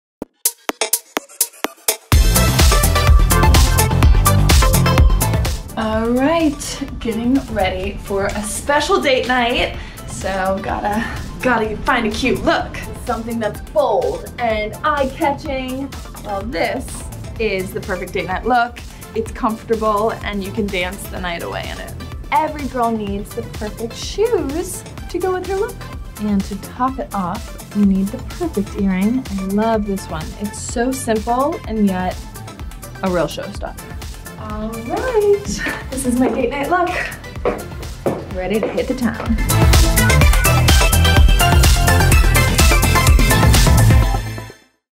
Getting Ready with Karlie Kloss
Go behind the scenes with Karlie Kloss as she gets ready for a night out in NYC!

Get her date night look in stores and online now:  short.url/aBcXyZ